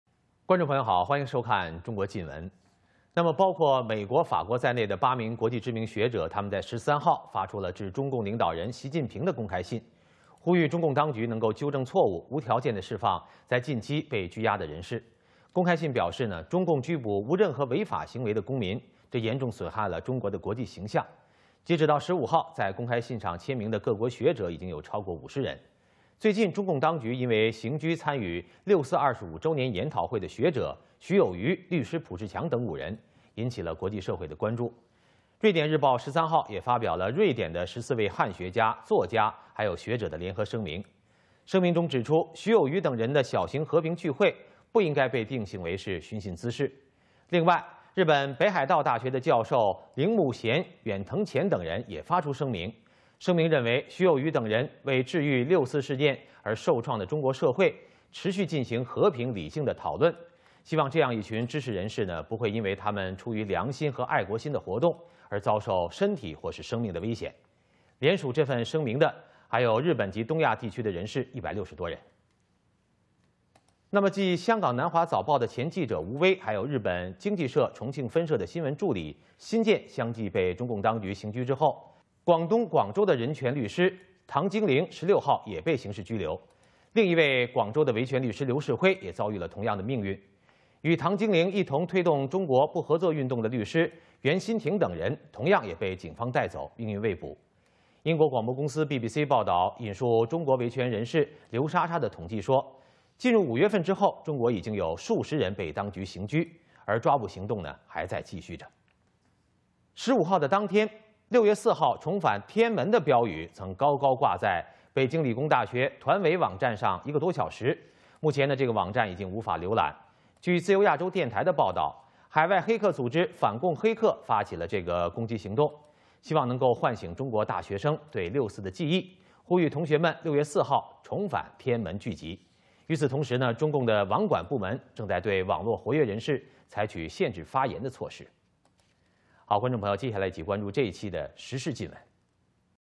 Release Chinese Citizens Arrested For no Reason!
Internationally renowned scholars from eight countries
including France and the U.S. issued an open letter on
May 13 to the Chinese leader Xi Jinping.

They called on the Chinese Communist Party (CCP) to
release those recently detained and correct the error.
The open letter said the arrest of citizens with no violations
seriously damages China's international image.
By May 15, more than 50 scholars from various countries
had signed the open letter.

The CCP recently detained 5 participants in the symposium
of the June 4th 25 anniversary including scholar Xu Youyu,
and lawyer Pu Zhiqiang, which caused international concern.

On May 13, Swedish Daily published a joint statement from
14 sinologists, scholars and writers from Sweden.
The statement said the small peaceful gathering of Xu Youyu
and others should not be characterized as trouble.

Japanese professor from Hokkaido University Suzuki Yin,
also issued a statement which said Xu Youyu and other
peoples' peaceful discussion is to cure the Chinese society
because of the June 4th incident.
They hoped such a group of professional people won't suffer
any harm because of conscientious and patriotic activities.
There are more than 160 signatories from Japan and East Asia
on the statement.

Chinese Communist Party Continues Arrests.
Dozens More People Are Detained.

After Wu Wei, former reporter of Hong Kong South China
Morning Post and Japanese Economic Society Chongqing
Branch information assistant Xin Jianxiang were arrested,
Guangzhou human rights lawyer Tang Jingling was arrested.
Another Guangzhou human rights lawyer Liu Shihui
also suffered the same experience.

Lawyer Yuan Xinting who promoted China non-cooperation
movement with Tang Jingling was also taken away by police.
There has been no information about them since.

The BBC quoted Chinese activist Liu Shasha's statistics
that dozens of people have been detained by the Chinese
Communist Party since May and arrests are continuing.

Return to Tiananmen Square on June 4th Posted
on University Website

The slogan of Return to Tiananmen on June 4th was on the
Beijing Institute of Technology Youth website on May 15
for more than one hour. The website cannot be accessed.

According to a RFA report overseas anti-communist hackers
launched this attack to remind Chinese college students of
June 4 and to gather at Tiananmen Square on June 4th.

So, the Chinese Communist Party network management
division restricted activists' speech on the network.